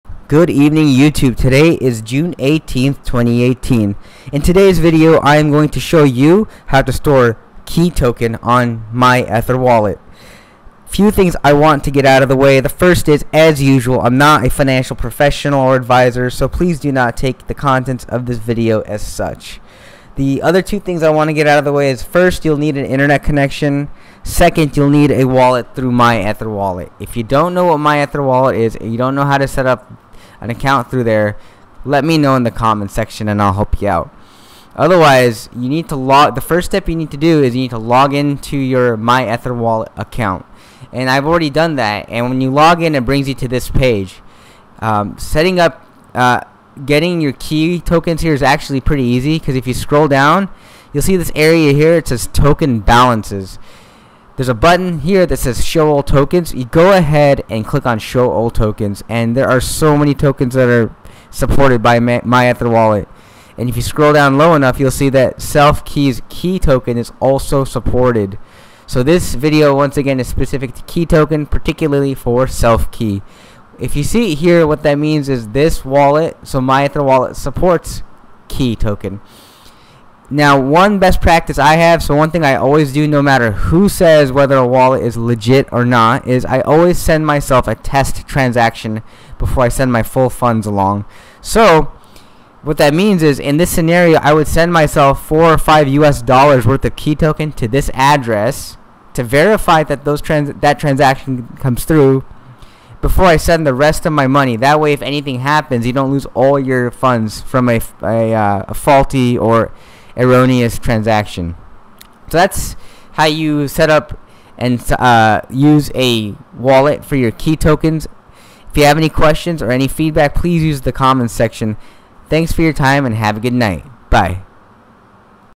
SelfKey Tokens (KEY) - Easy Wallet Setup - MyEtherWallet
Video Summary:
The purpose of this video is to show you how to quickly and easily create a wallet for storing your KEY tokens using MyEtherWallet.

Have questions? Comment them in.